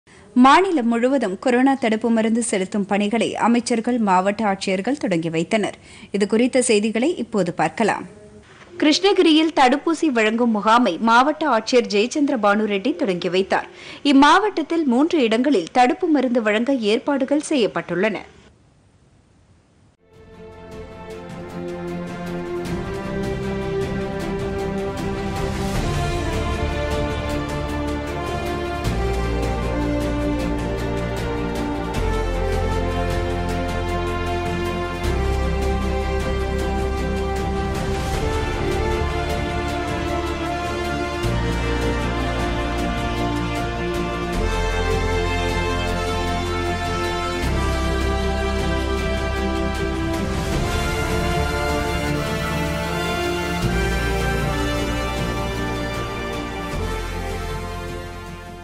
கிருஷ்ணகிரியில் கொரோனா தடுப்பூசி
மாநிலம் முழுவதும் கொரோனா தடுப்பு மருந்து செலுத்தும் பணிகளை அமைச்சர்கள், மாவட்ட ஆட்சியர்கள் தொடங்கி வைத்தனர்